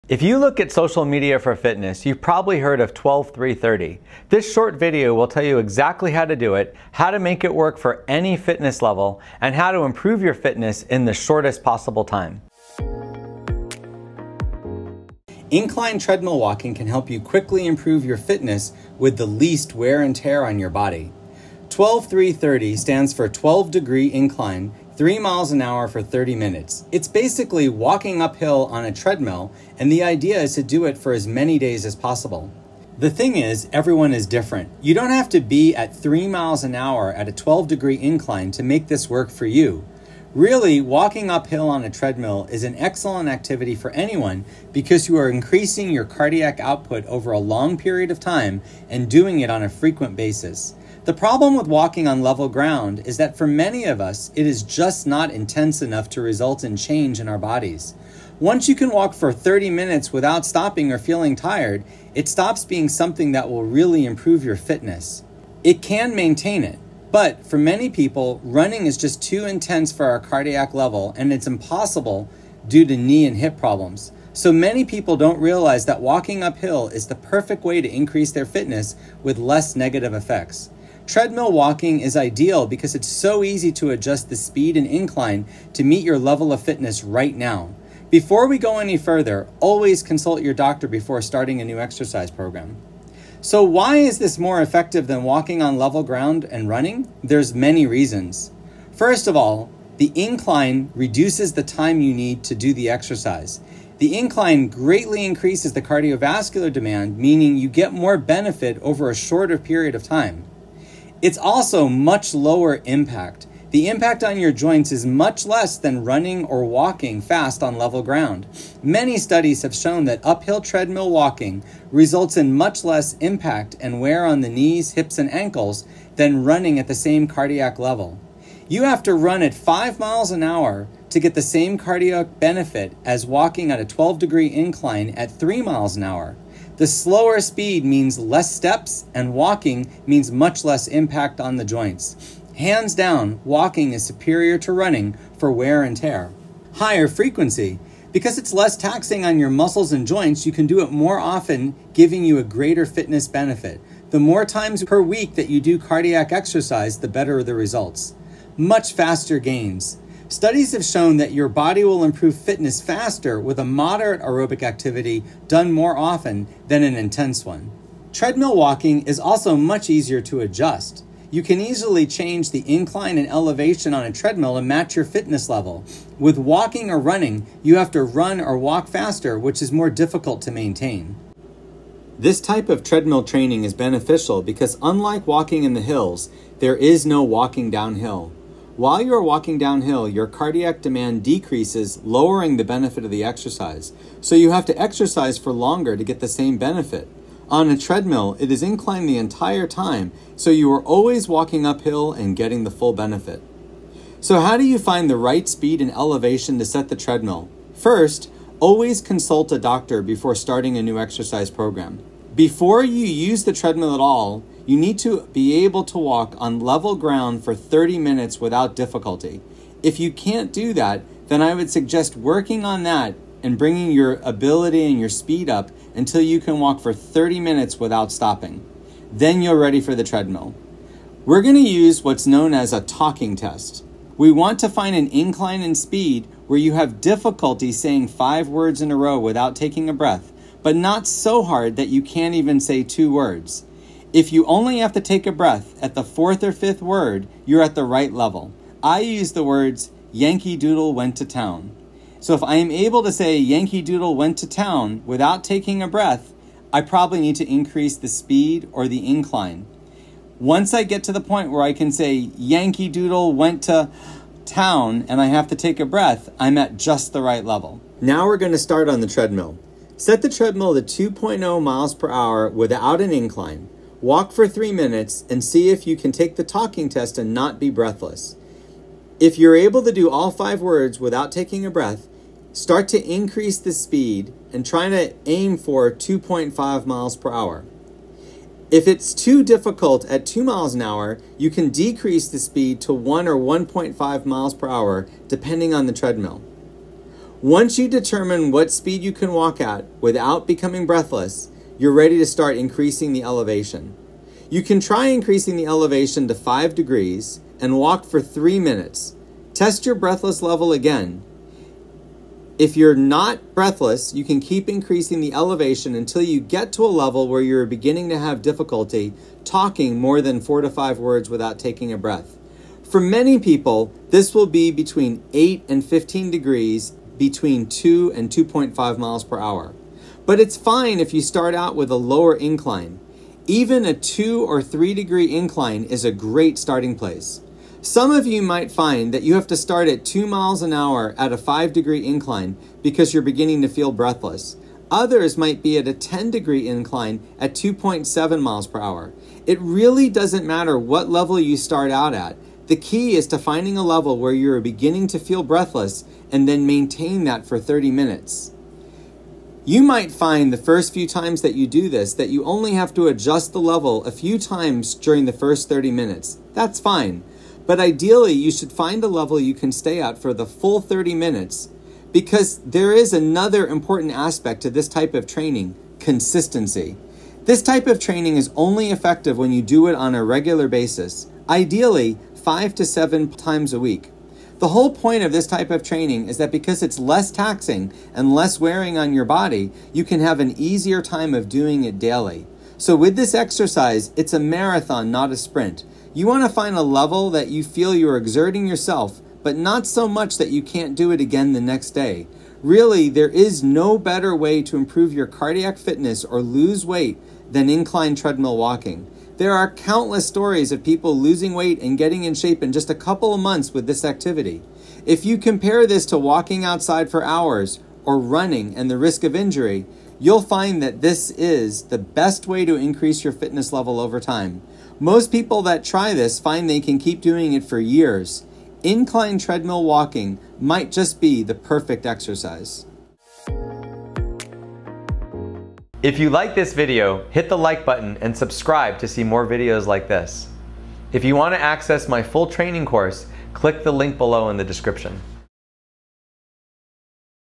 12:3:30. The Best way to increase your FITNESS!
There is no better way to lose weight, increase your endurance, or improve your fitness than the 12:3:30 workout. This video will explain why its the BEST exercise and how to make it work for you.